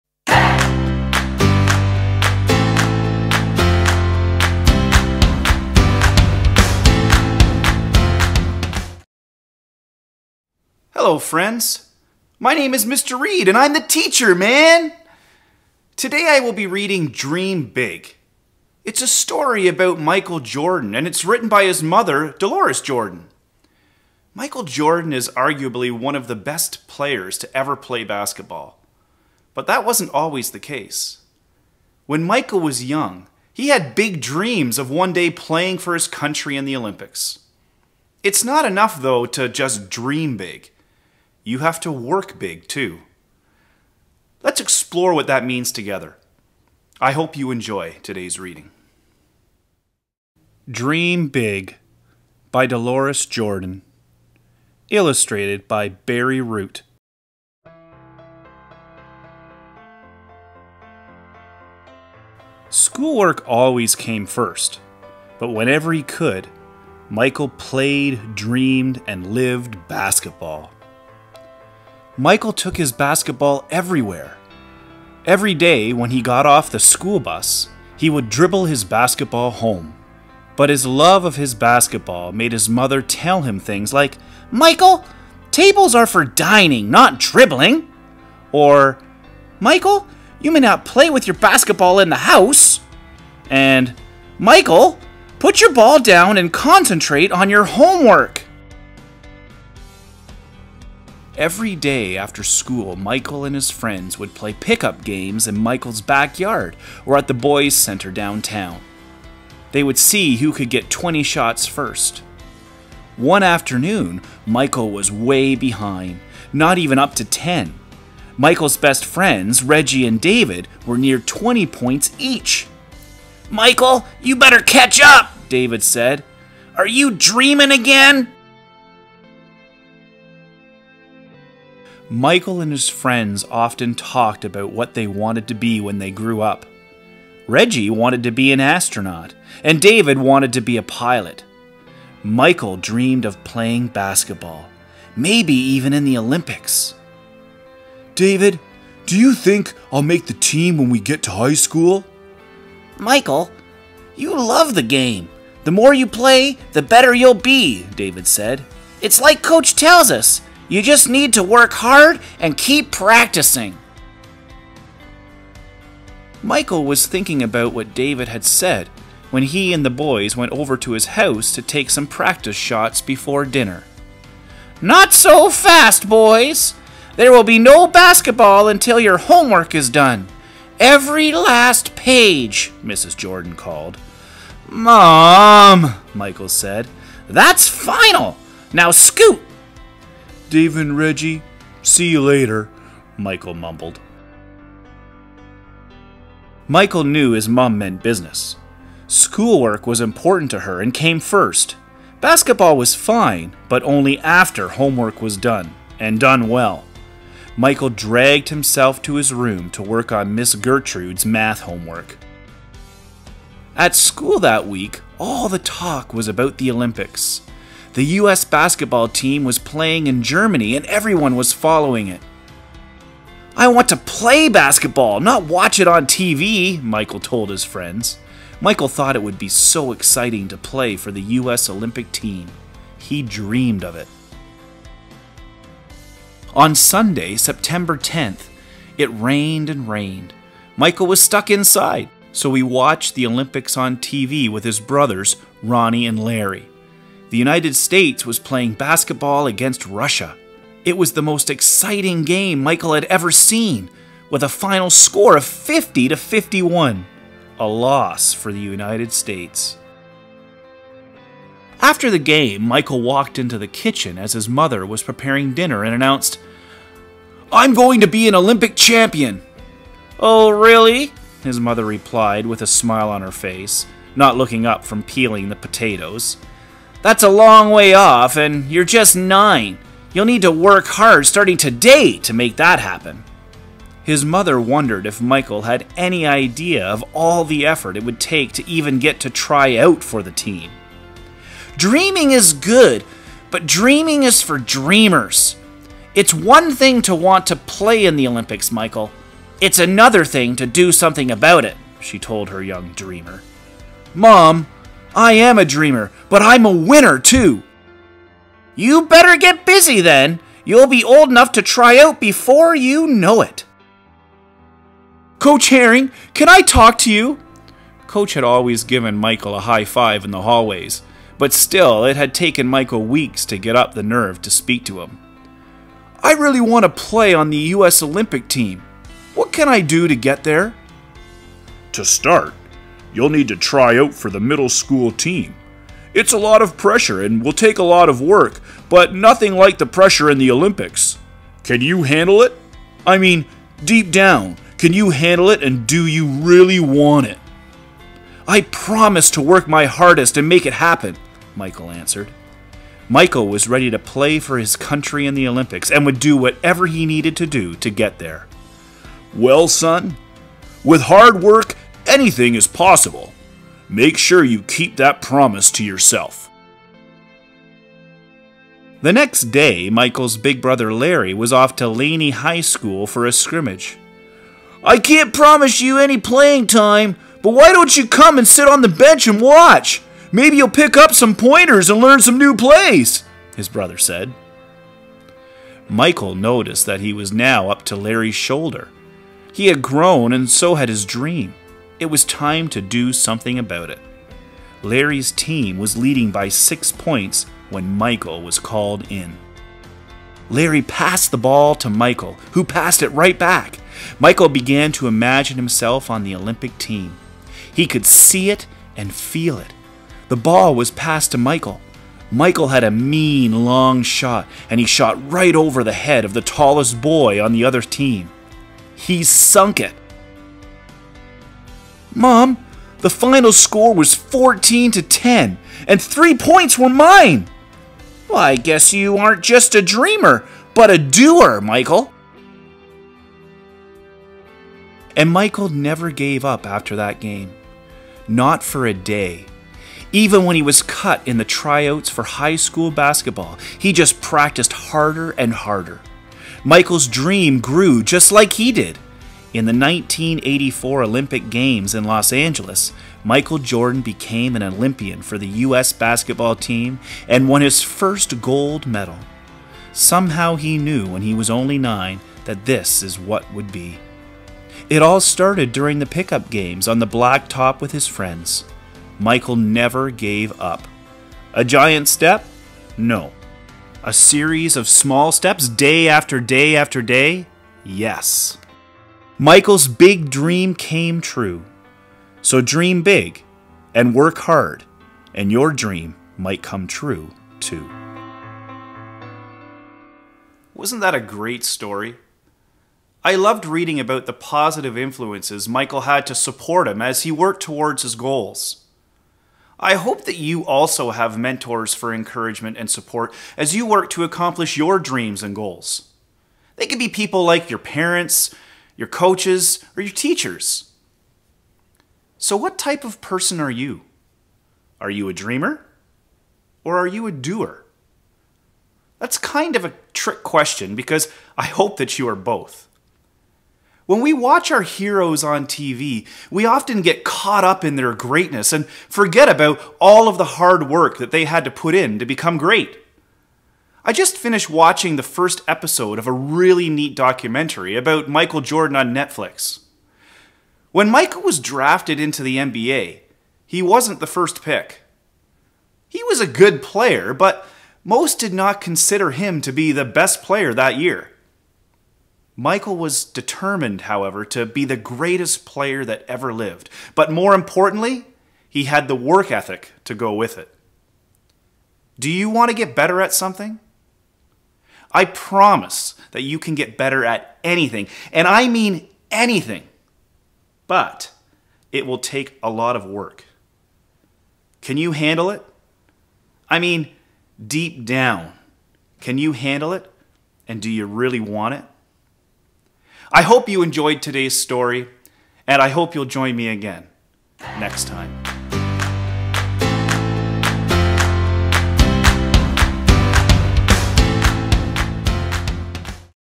Dream Big
Do you dream big? I hope that you do. It’s not enough, though, to just dream big… you need to work big too. Let’s explore what that means in today's read aloud together.